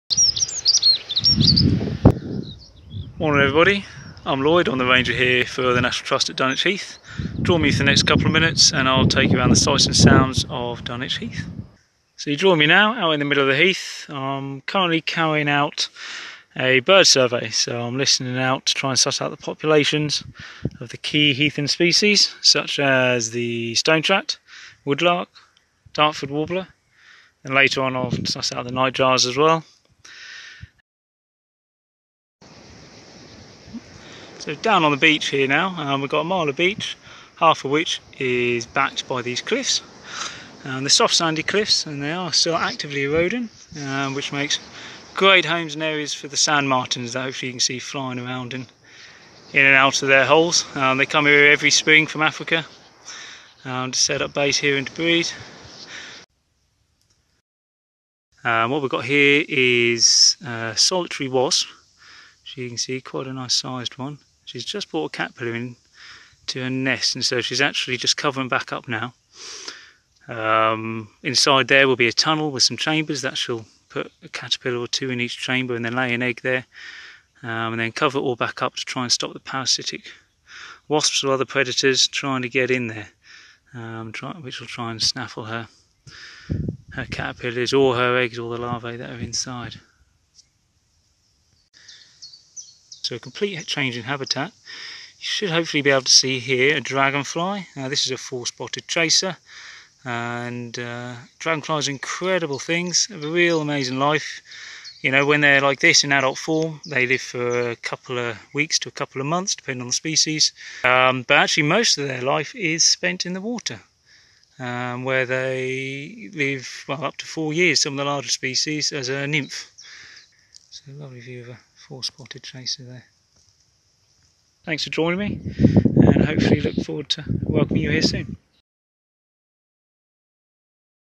Wildlife encounters at Dunwich Heath - Ranger Diary
This ranger diary comes to you from Lloyd at Dunwich Heath, who takes us to different parts of the reserve and shares some of his wildlife encounters with you. From sand martins in the cliffs to dragonflies at the water's edge, he doesn't have to look far before spotting something of interest.